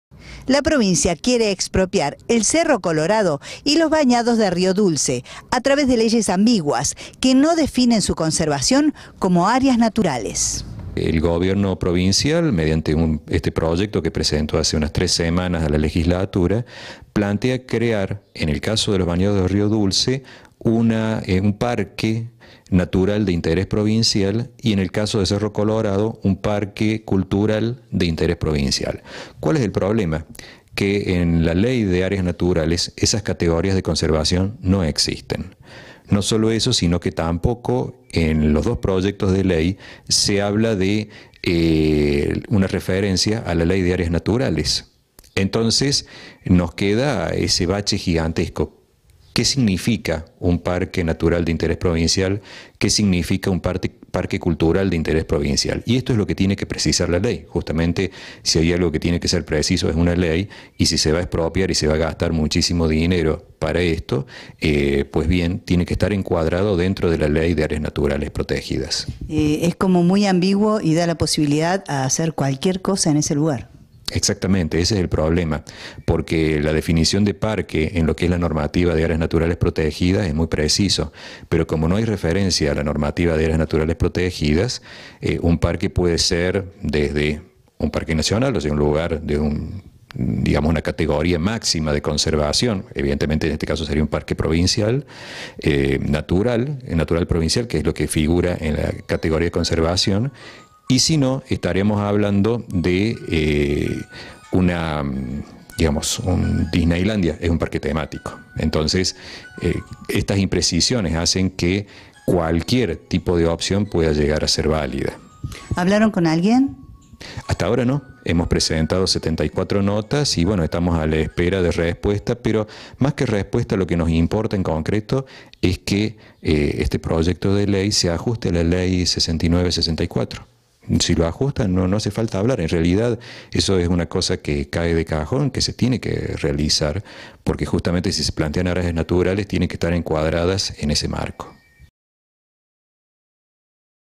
"Puede ser desde un parque provincial a un Disneylandia"
Federico Kopta criticó la ambigüedad del proyecto provincial de expropiación del Cerro Colorado y los Bañados de Río Dulce.